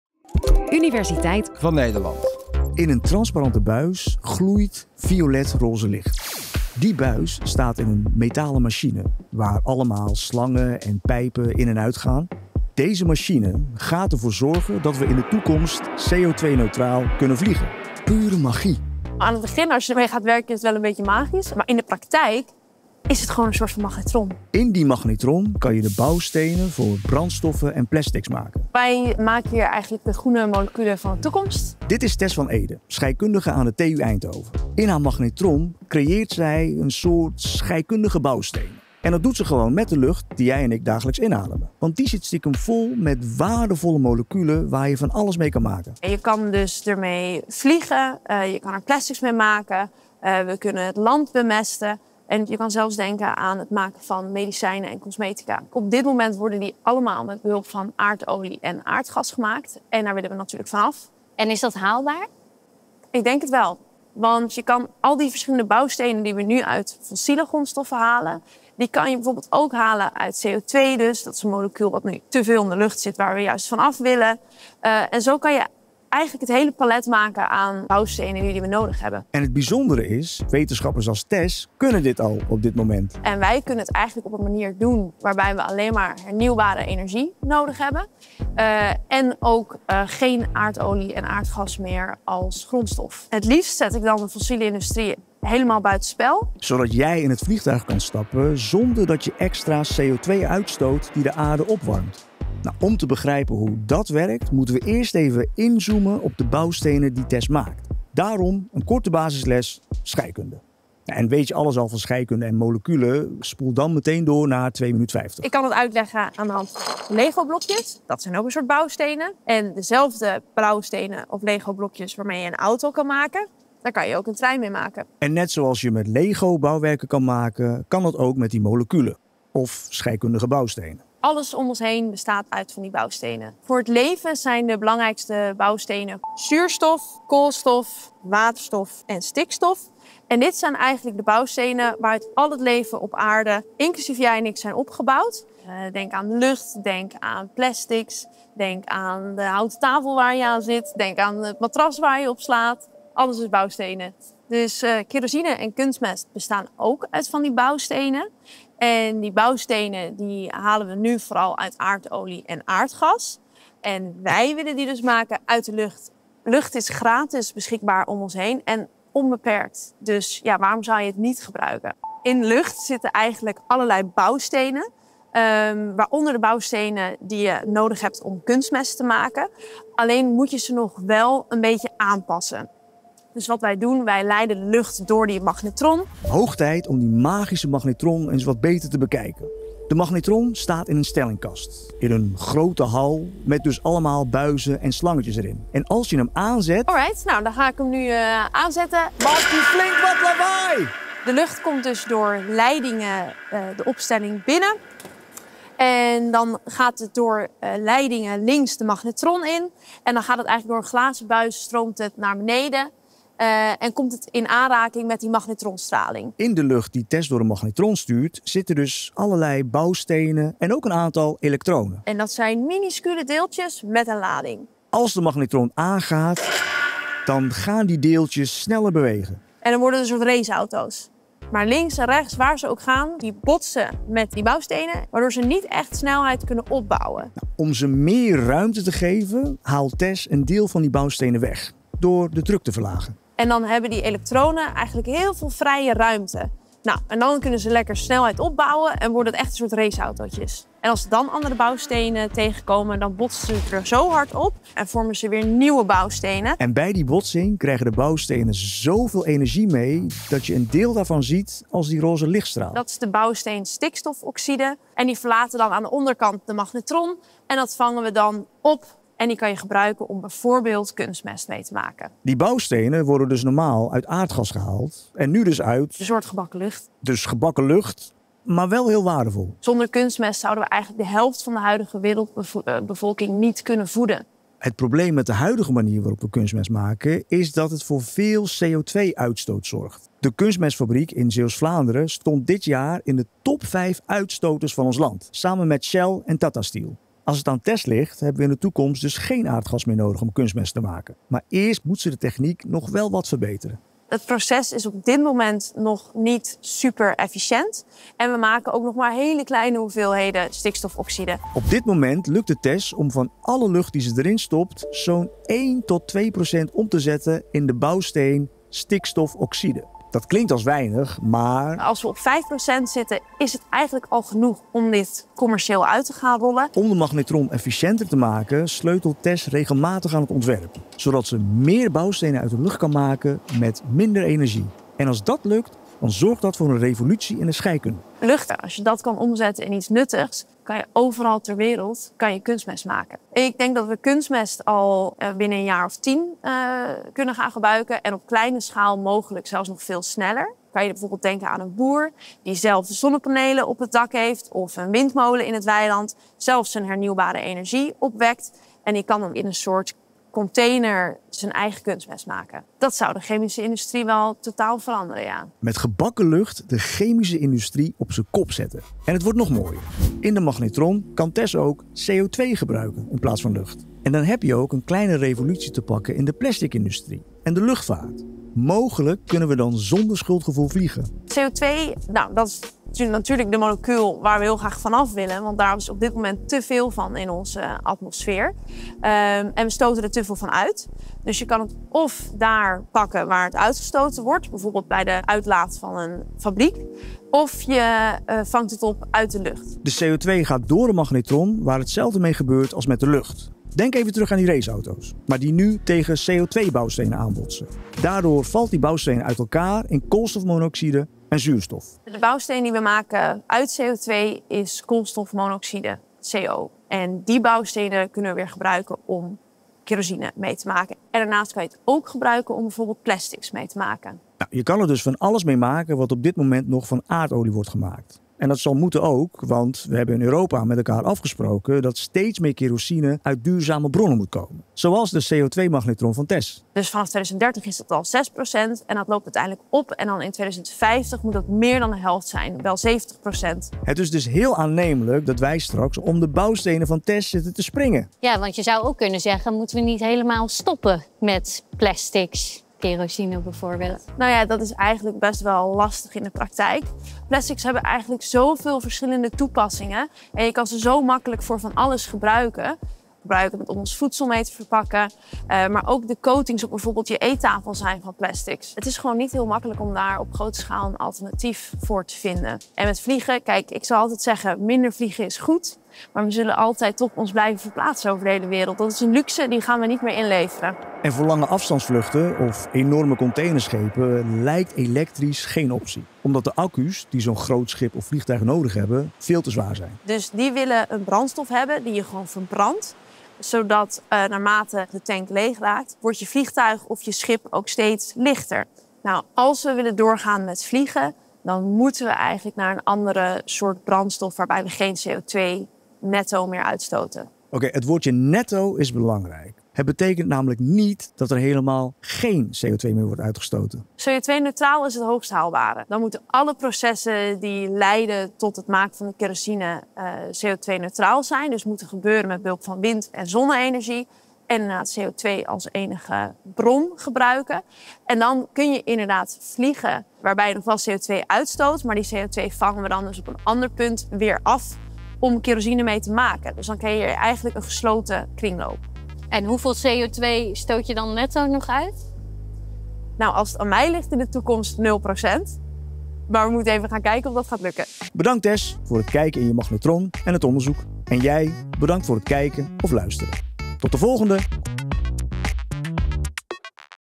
Revolutie in de lucht: CO2-vrij op vakantie. Kan het echt?
Het klinkt allemaal als verre toekomstmuziek, misschien zelfs onmogelijk, maar het is een stuk dichterbij dan je denkt. Scheikundige Tess van Eeden (TU/ Eindhoven & DIFFER) maakt in haar lab bouwstenen voor brandstof en plastics, uit de lucht. Gewoon de lucht die we dagelijks inademen.

Waarom Tess dit doet? Zodat we in de toekomst helemaal geen aardolie of aardgas meer nodig voor onze vluchten, kunstmest en plastics en dag kunnen zeggen tegen een groot deel van het CO₂ probleem! Benieuwd of jij straks zonder schuldgevoel in het vliegtuig kunt stappen? Tess vertelt je er alles over in deze video.

0:00 Hoe kan je CO₂-neutraal vliegen?
1:44 Basisles scheikunde
3:05 Hoe maak je de bouwstenen voor kerosine en kunstmest?
4:36 Waarom hebben we kunstmest nodig?
6:10 Hoe maak je kerosine uit CO₂?
7:04 Hoe kansrijk is dit project?

Wil je ons iets vragen? Of heb je een opmerking? Dat kan via dit formulier